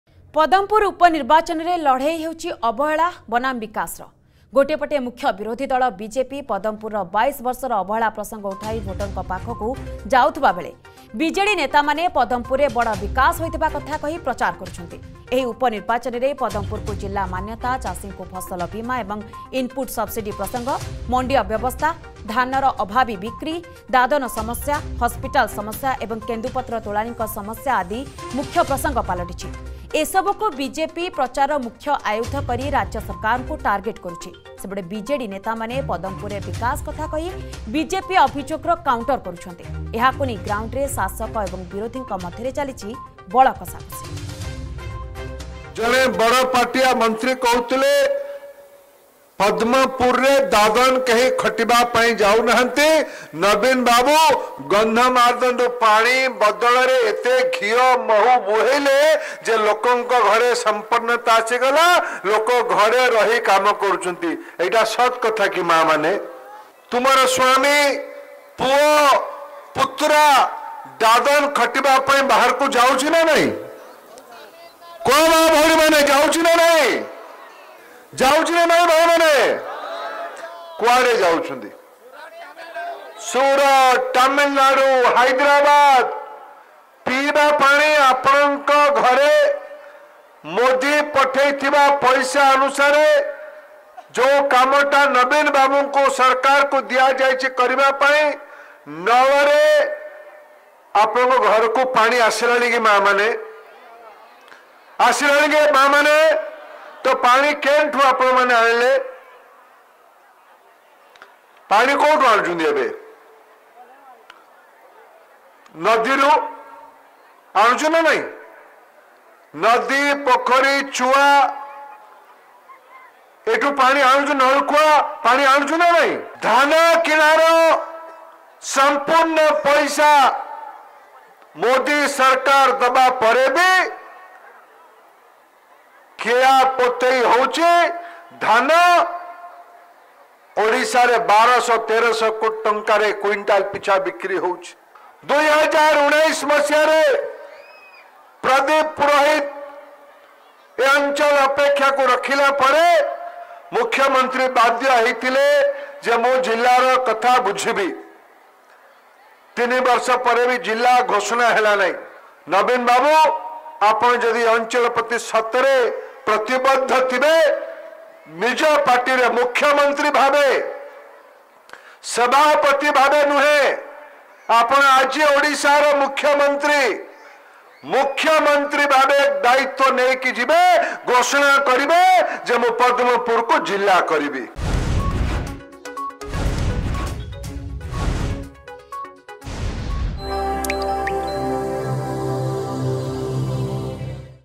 Padampur By Election | 22 ବର୍ଷର ଅବହେଳା ବନାମ
22 ବର୍ଷର ଅବହେଳା ବନାମ
ପଦମପୁରର ବଡ଼ ବିକାଶ । କୃଷି, ସ୍ବାସ୍ଥ୍ୟ, ଜିଲ୍ଲା ମାନ୍ୟତା, ଦାଦନ ଓ କେନ୍ଦୁପତ୍ର ତୋଳାଳିଙ୍କ ସମସ୍ୟାକୁ ପ୍ରସଙ୍ଗ କରି ଭୋଟ୍ ମାଗୁଛି ବିଜେପି ।